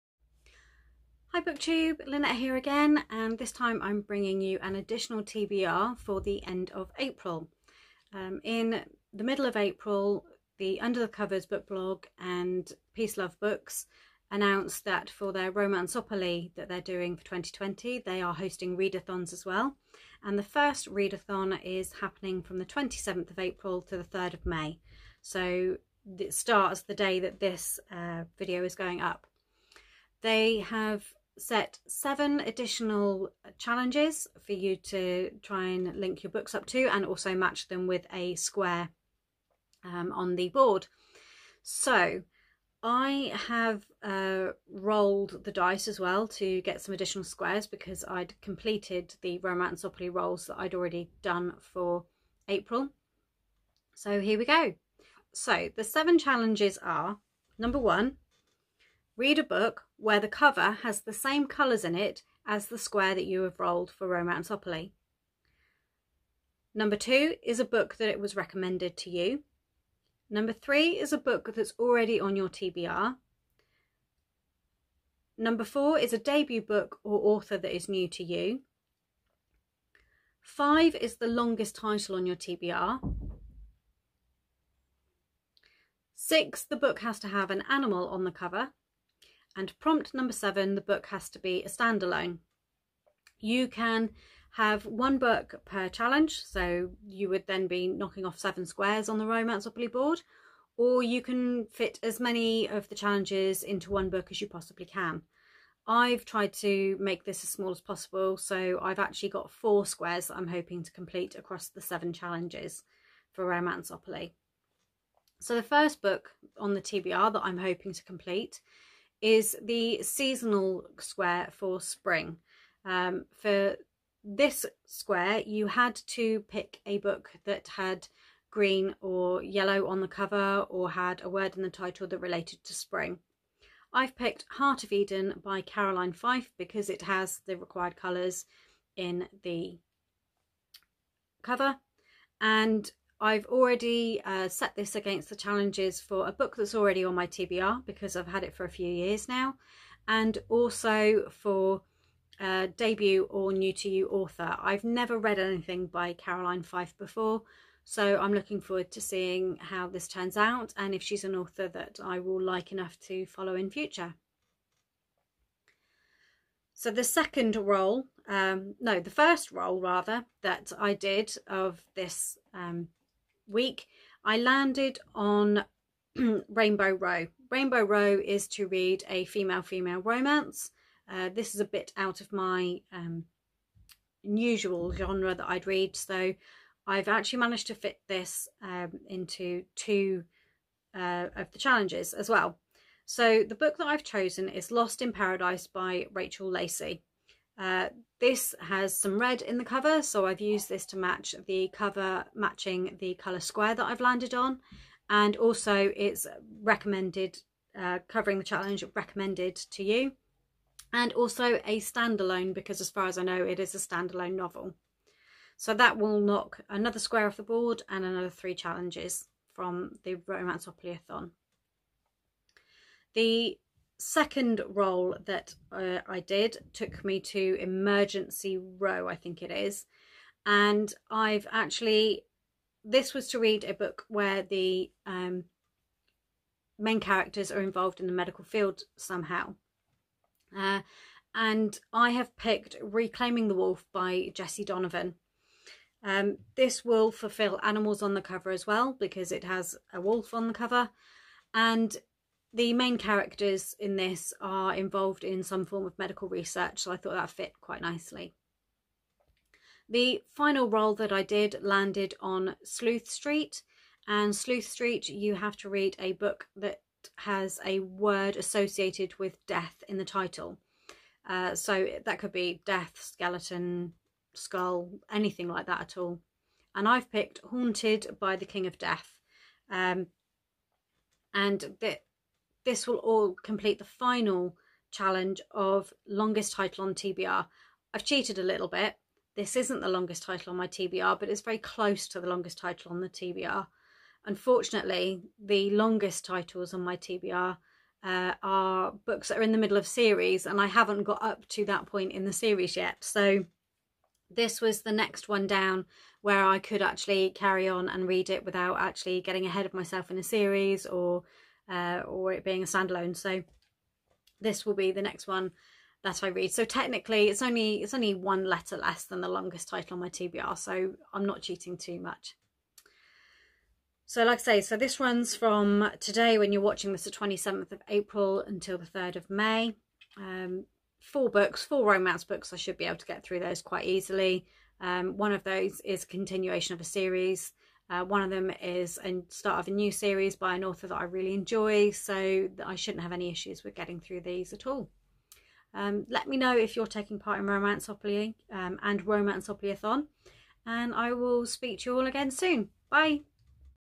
Romanceopolyathon TBR April 2020
Hi All in this video I confirm the books I am reading for the Romanceopoly Readathon which runs from 27/03/2020 to 03/05/2020.

Challenges
1. The cover has the same colour as the Romanceoploy square you are reading it for
2. A book that was recommended to you
3. A book that was already on your TBR
4. A debut book or author that is new to you
5. A book with the longest title on your TBR
6. A book that has an animal on the cover
7. A book that is a standalone